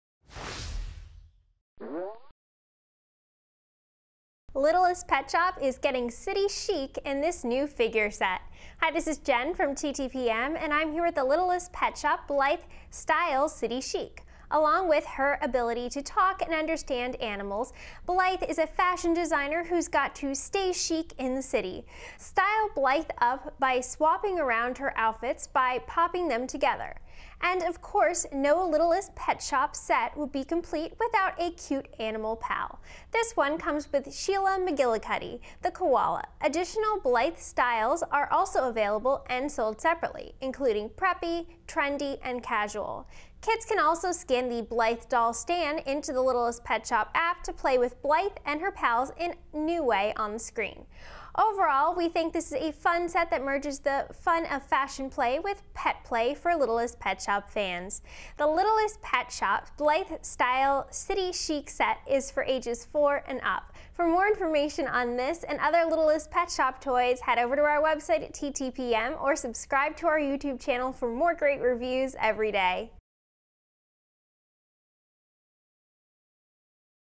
Littlest Pet Shop Blythe Style City Chic from Hasbro
The Littlest Pet Shop Blythe Style City Chic set is a small doll playset that lets kids swap around Blythe's outfits. For full review and shopping info

Product Info:
The Littlest Pet Shop Blythe Style City Chic playset lets kids swap and style the Littlest Pet Shop character Blythe who along with her ability to talk and understand animals, is a fashion designer. Style Blythe by swapping around her outfits. The doll breaks down into three pieces: head, top, and bottom. It comes with two top halves and two bottom halves, so kids can mix-and-match Blythe's outfits. She also comes with two small accessories to complete her look. Also included is one Littlest Pet Shop animal pal: Sheila MacGillicuddy the koala.